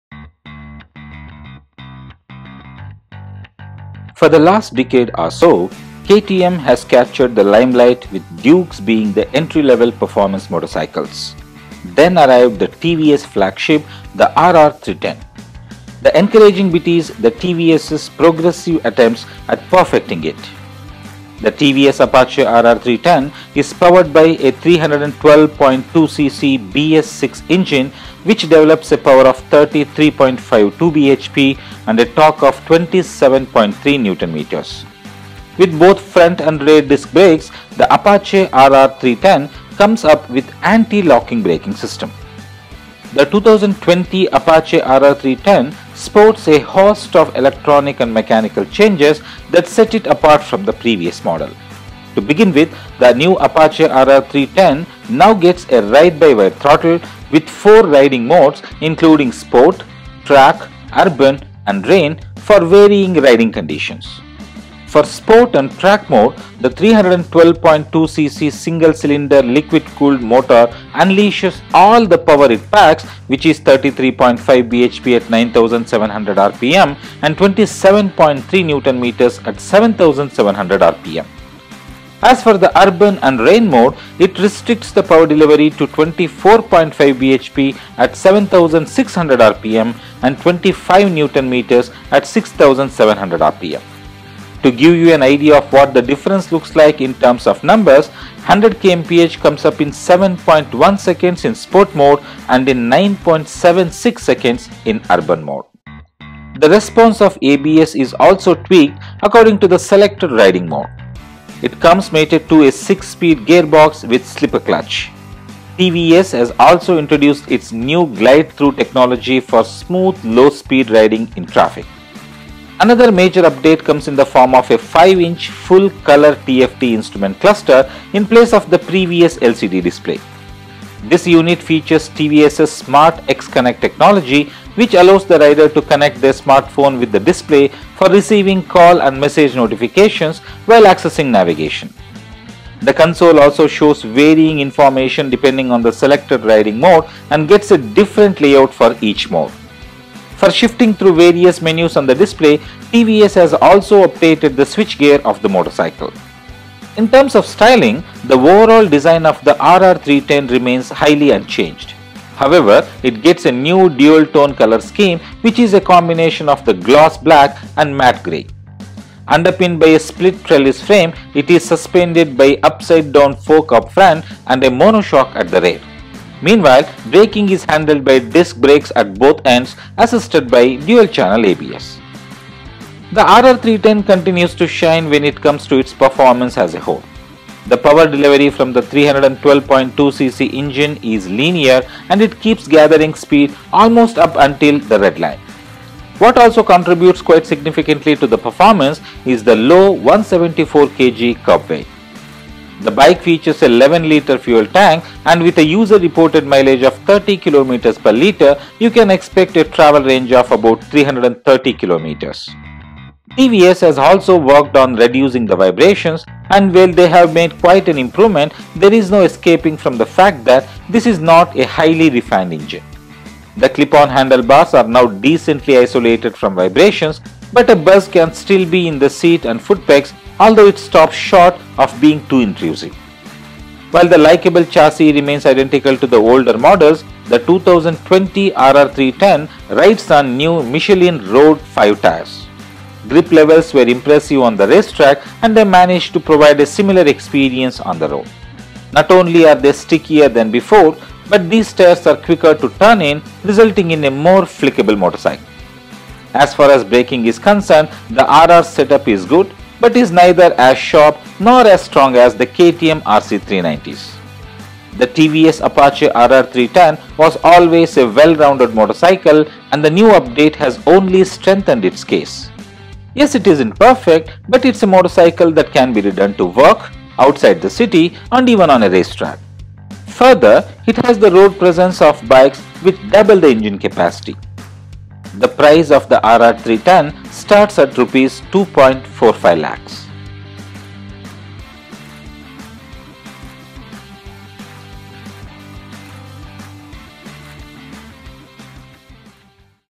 TVS Apache RR 310: Overview
In this video, we give a quick review of the TVS Apache RR 310.